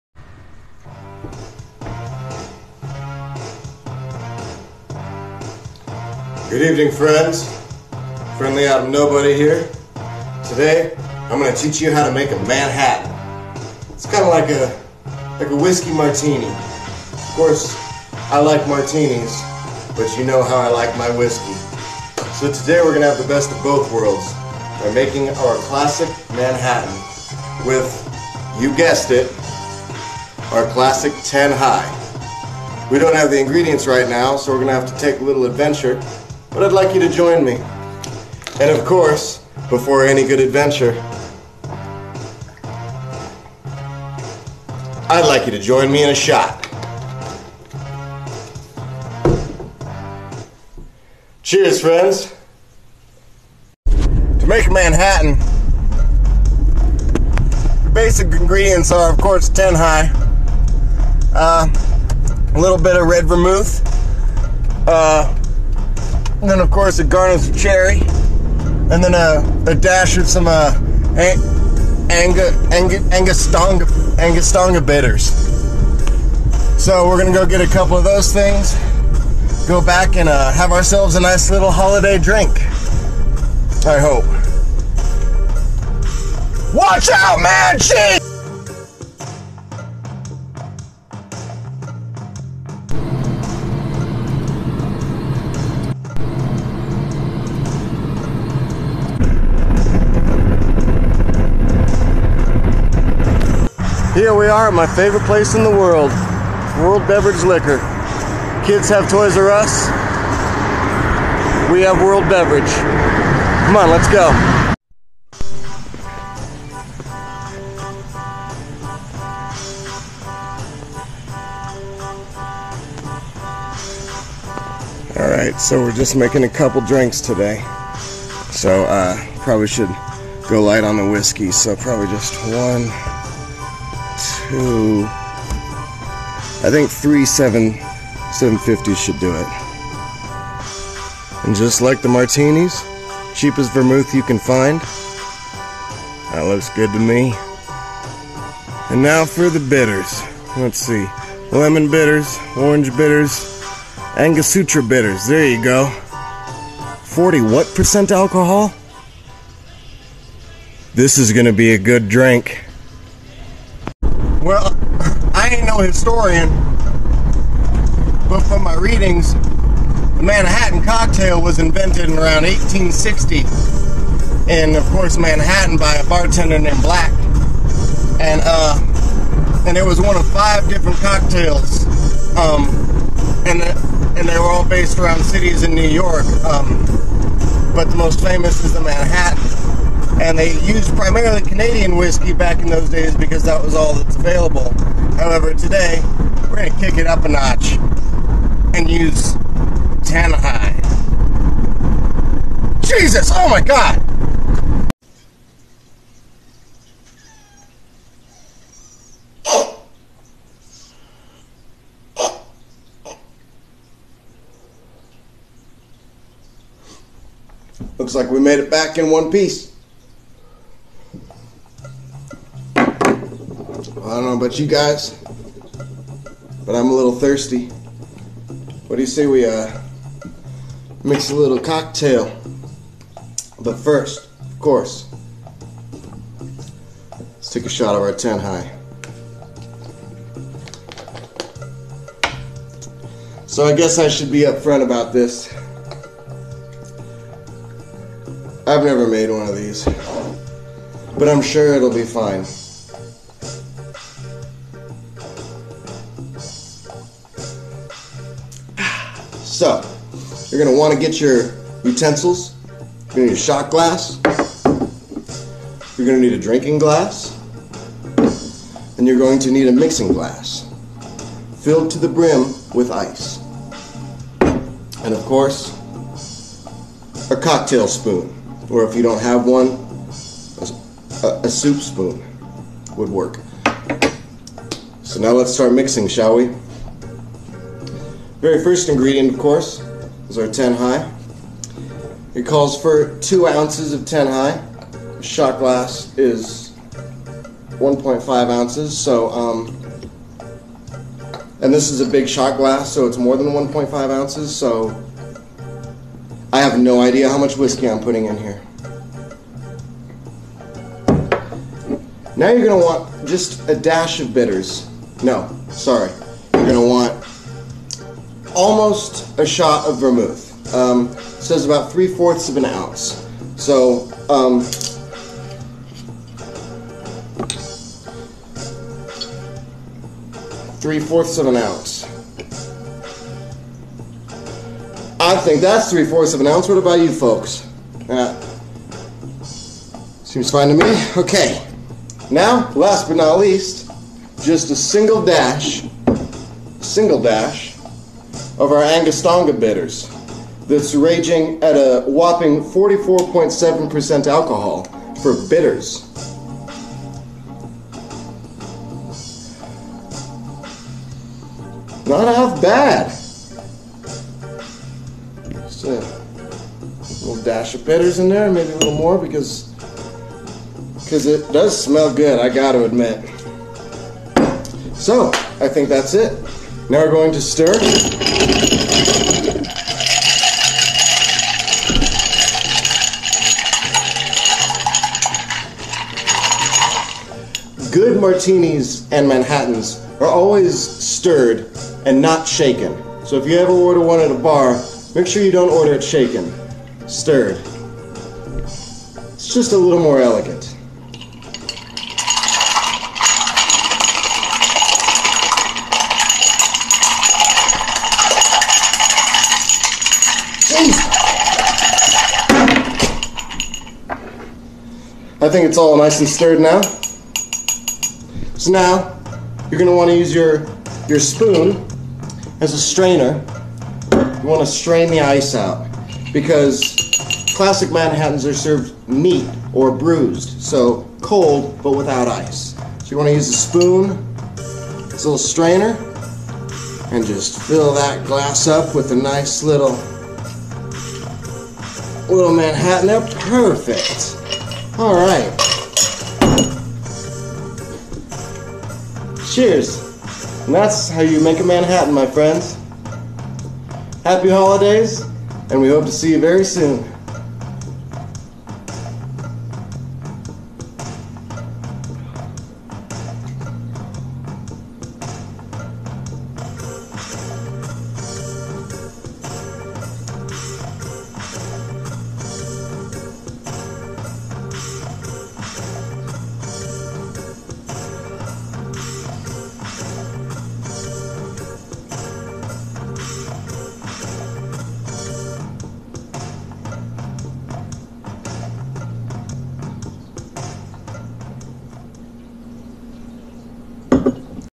Adam Eats Episode 7: Manhattan
Adam Nobody teaches you how to make a Manhattan... And learns himself.